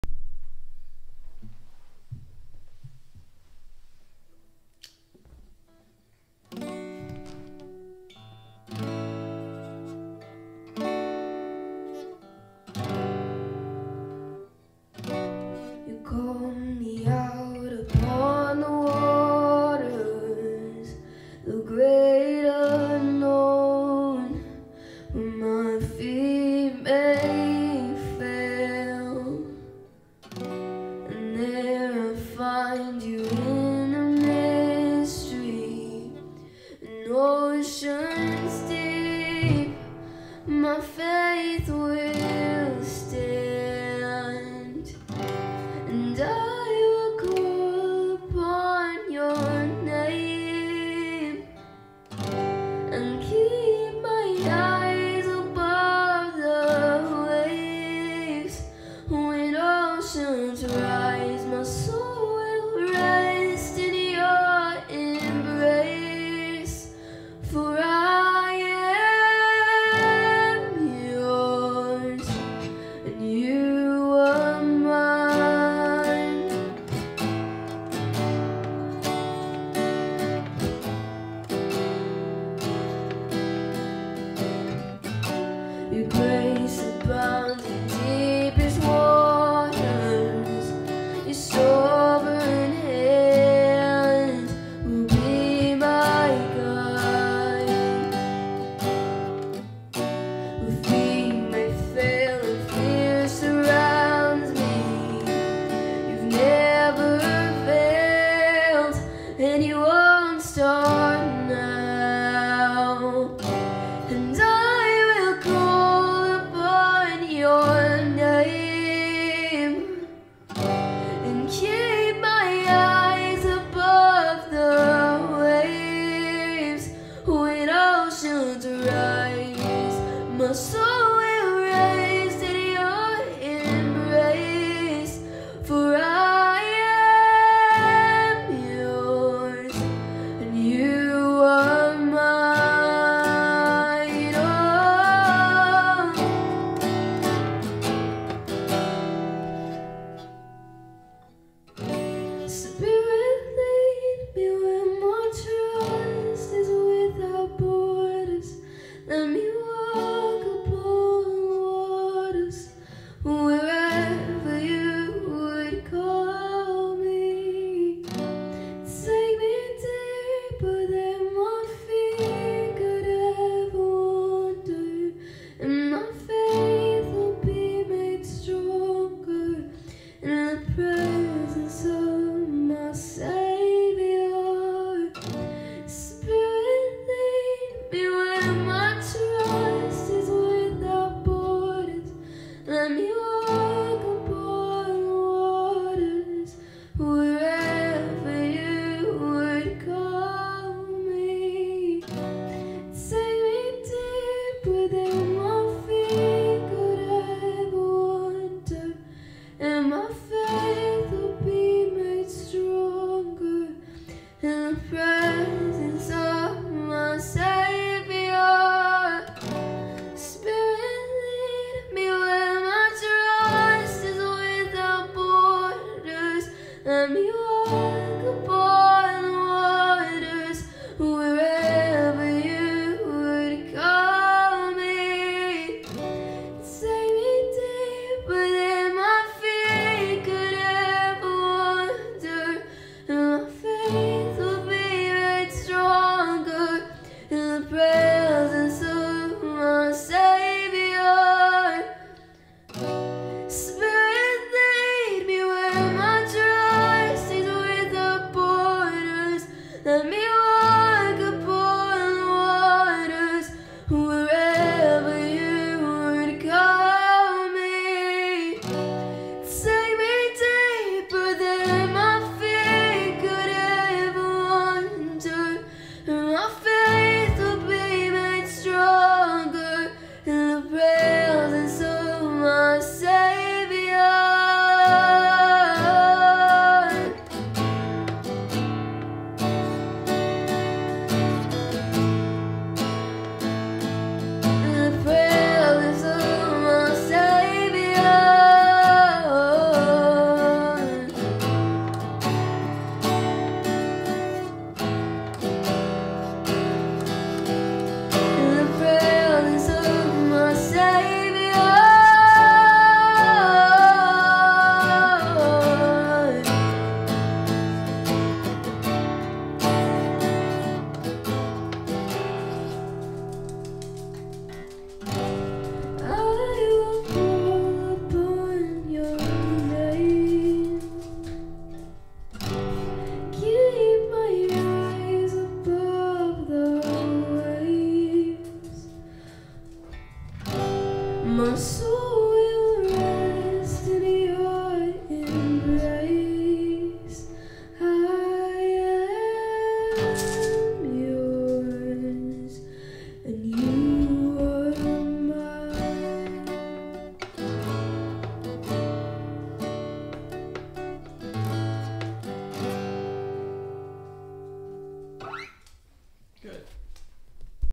Acoustic! Practicing
A friend of my dads loaned me this Sigma Guitar to practice with. It’s really nice and I love the sound.